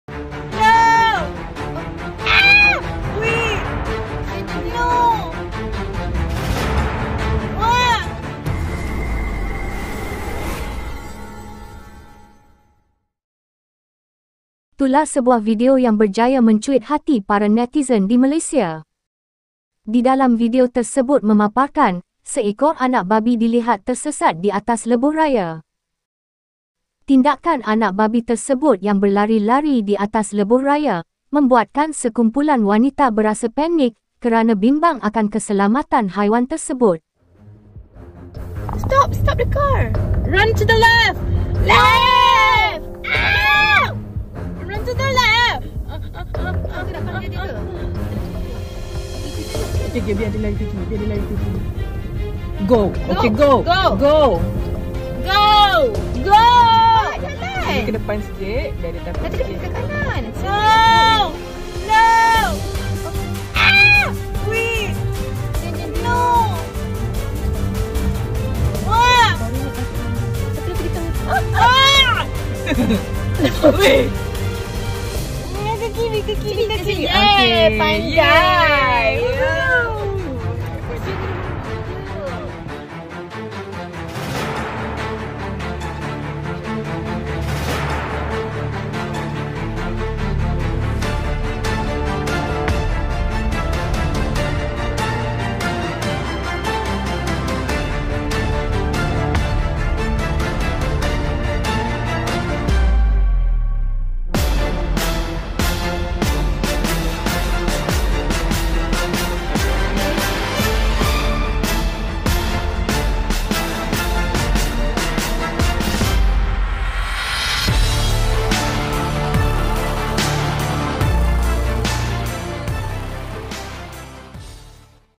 Wanita berasa panik terserempak anak babi atas lebuhraya
DETIK CEMAS DAN PANIK terserempak anak babi atas lebuhraya memaparkan rakaman yang dikongsikan oleh sekumpulan wanita yang memaparkan seekor anak babi tersesat dan berada di atas lebuhraya.